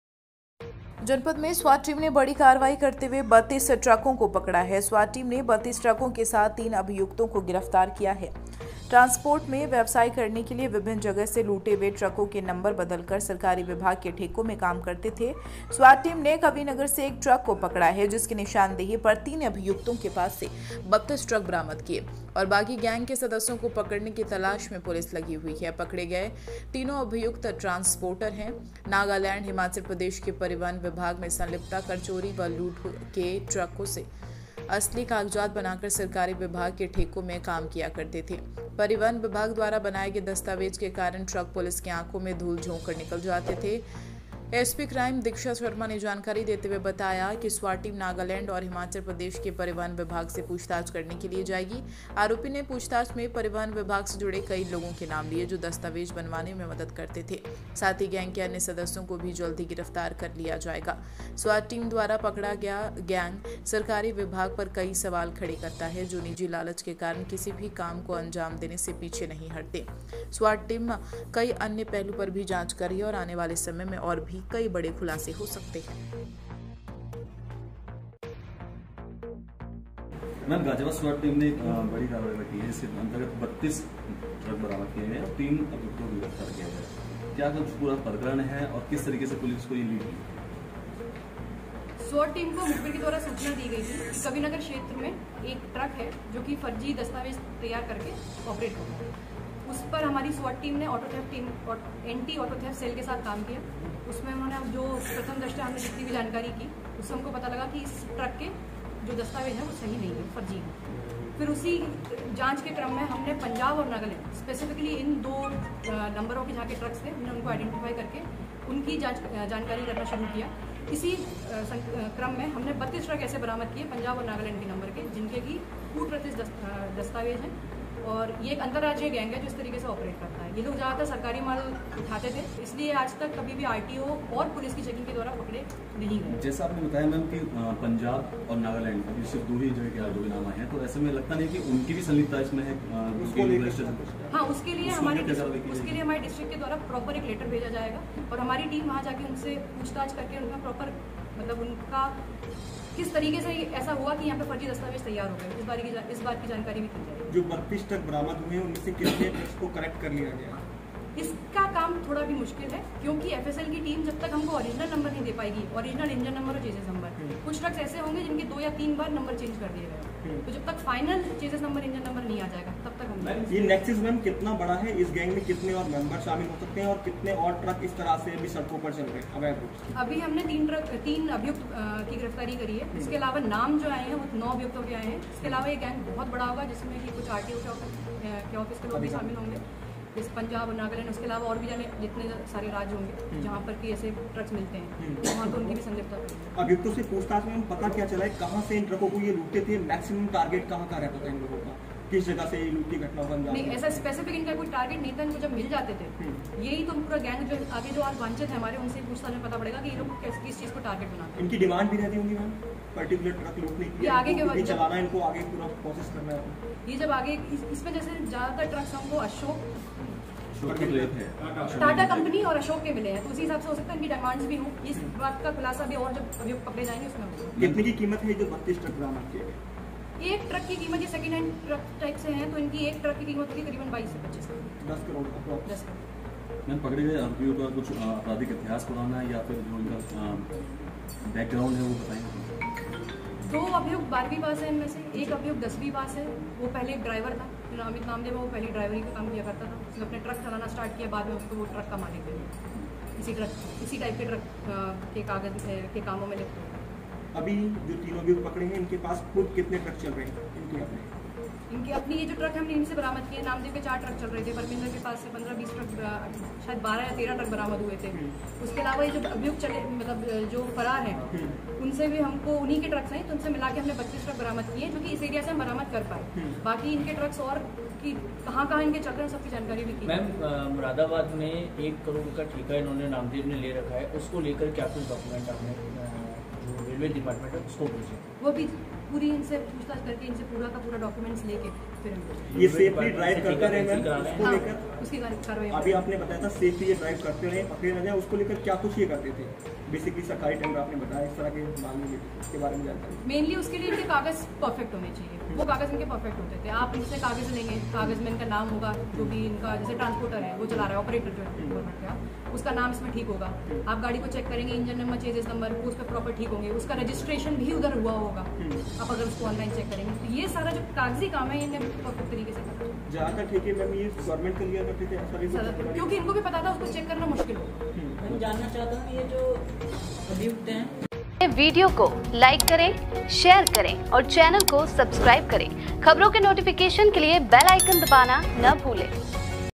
चोरी किए गए ट्रकों के नंबर बदलकर सरकारी विभाग के ठेकों में काम करवाते थे ट्रांसपोर्टर, 3 गिरफ्तार
गाजियाबाद। जनपद में स्वाट टीम ने बड़ी कार्रवाई करते हुए 32 ट्रकों को पकड़ा है। स्वाट टीम ने 32 ट्रकों के साथ तीन अभियुक्तों को भी गिरफ्तार किया है। ट्रांसपोर्ट में व्यवसाय करने के लिए विभिन्न जगह से लुटे हुए ट्रकों के नंबर बदलकर सरकारी विभाग के ठेकों में काम करते थे। स्वाट टीम ने कविनगर से एक ट्रक को पकड़ा। जिसकी निशानदेही पर तीन अभियुक्तों के पास से 32 ट्रक बरामद किए हैं और बाकी गैंग के सदस्यों को पकड़ने की तलाश में पुलिस लगी हुई है। पकड़े गए तीनों अभियुक्त ट्रांसपोर्टर है।
नागालैंड और हिमाचल प्रदेश के परिवहन विभाग से संलिप्ता कर के चोरी व लुट के ट्रकों के असली कागजात बनाकर सरकारी विभाग के ठेकों में काम किया करते थे। परिवहन विभाग द्वारा बनाए गए दस्तावेज के कारण ट्रक पुलिस की आंखों में धूल झोंक कर निकल जाते थे। एसपी क्राइम दीक्षा शर्मा ने जानकारी देते हुए बताया कि स्वाट टीम नागालैंड और हिमाचल प्रदेश के परिवहन विभाग से पूछताछ करने के लिए जाएगी। आरोपियों ने पूछताछ में परिवहन विभाग से जुड़े कई लोगों के नाम लिए हैं जो दस्तावेज बनवाने में मदद करते थे। साथ ही गैंग के अन्य सदस्यों को भी जल्द ही गिरफ्तार कर लिया जाएगा।
स्वाट टीम द्वारा पकड़ा गया गैंग सरकारी विभाग पर कई सवाल खड़े करता है। जो निजी लालच के कारण किसी भी काम को अंजाम देने से पीछे नहीं हटते हैं। स्वाट टीम कई अन्य पहलू पर भी जांच कर रही है और आने वाले समय में और भी कई बड़े खुलासे हो सकते हैं।